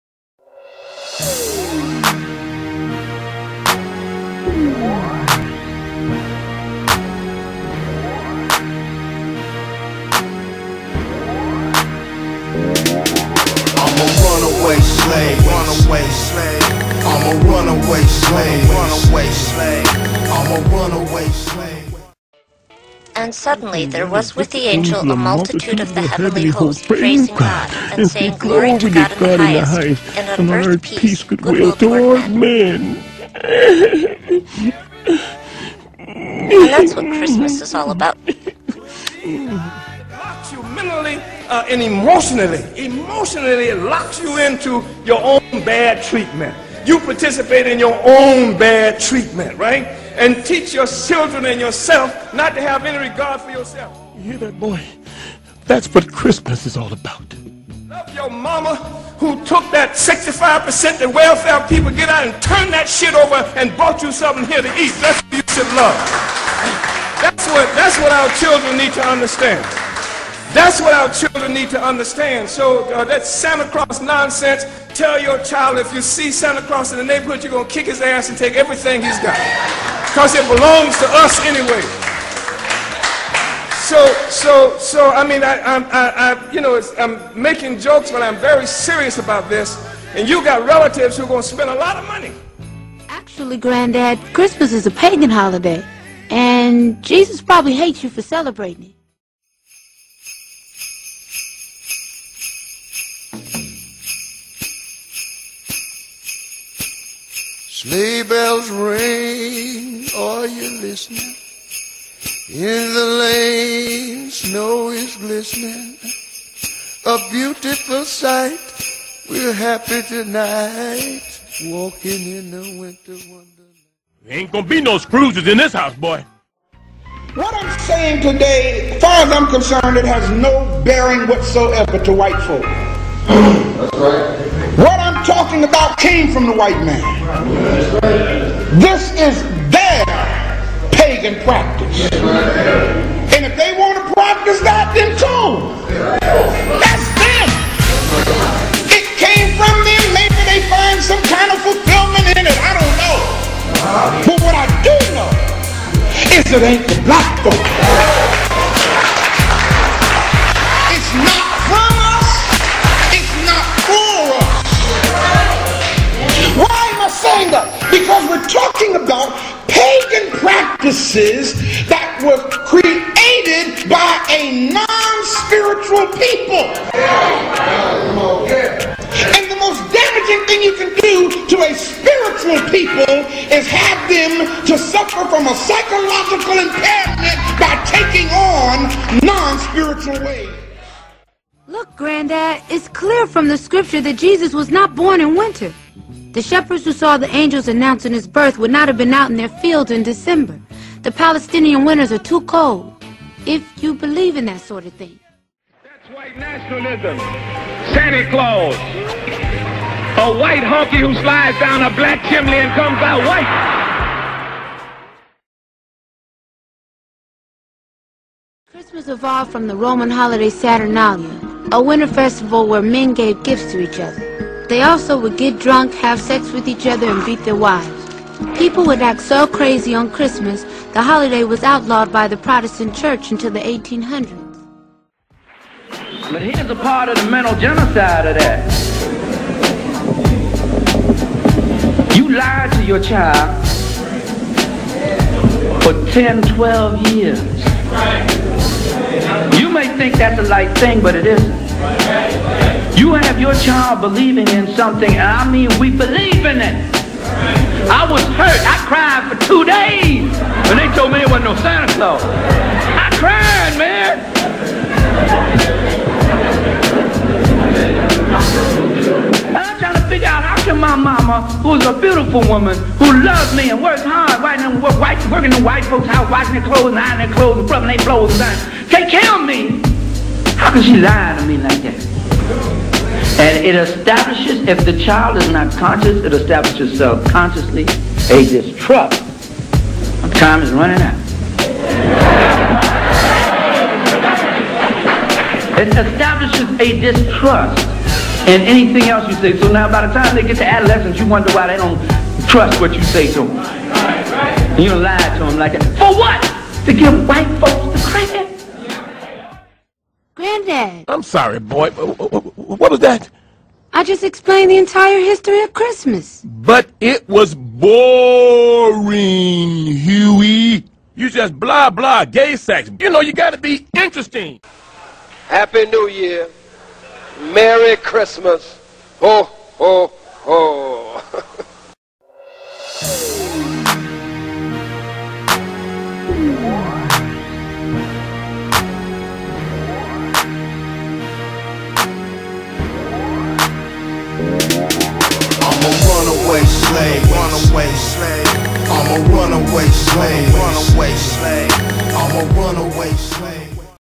Merry Krazymess
Afrikans!!!

We celebrate da christ-mass & otha euro peon horrordayz every yr & in doin sooo We perpetuate Our Own menticide... menticide occurs when We willingly think & act out of some1 else's interpretation of reality tooo dere benefit & against Our survival. It is a state of subtle insanity which over da last few hundred years has come to characterize more & more Afrikans globally...

We canNOT be some1 else & Ourselves @ da same TIME especially when dat some1 else is hell-bent (b/c dey are da devil) on destroyin Us..!

as We kno problems & solutions work hand in hand... w/out knowin somethang is wrong or if aware why what is wrong is wrong We canNOT implement ideas or programs dat wil help Us solve Our problems to Our advantage...